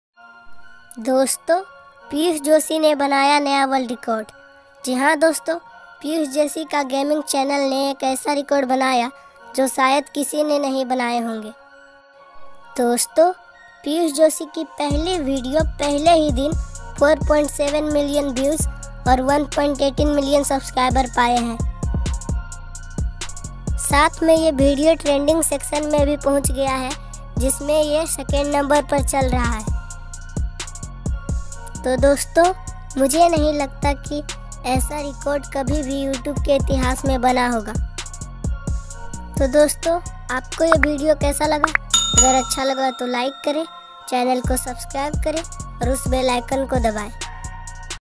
Piyush Joshi gaming channel record|saurav joshi|world record
Piyush Joshi gaming channel record|piyush Joshi gaming|saurav joshi

about video:-

Piyush Joshi gaming channel ने बनाया नया world record piyush Joshi gaming channel के पहले video पहले ही दिन 4.7 million views और 1.12 subscriber पाया है।

cover topic:-

piyush Joshi gaming record
world record
saurav Joshi vlog
saurav Joshi
piyush Joshi record